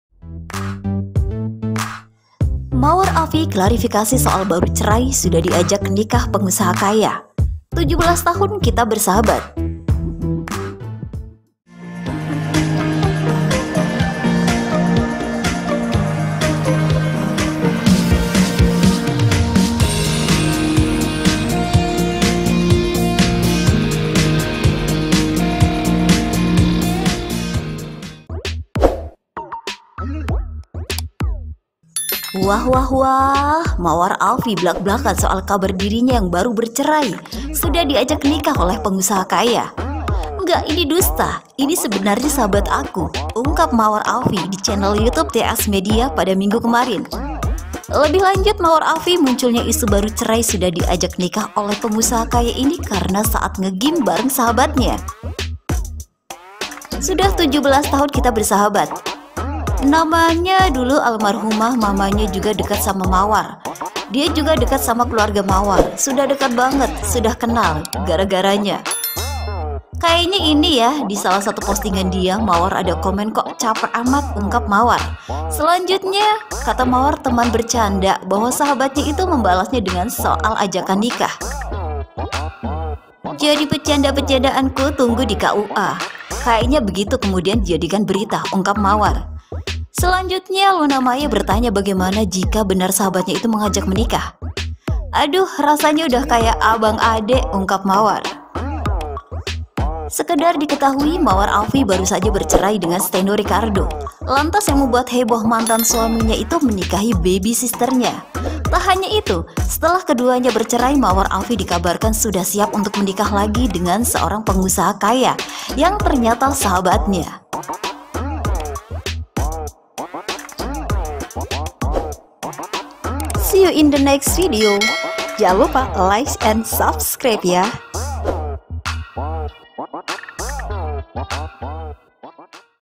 BARU AJA CERAI‼️ SUDAH DIAJAK NIKAH PENGUSAHA KAYA ~ Begini Klarifikasi Mawar AFI..
Sumber:IsuBogor.Com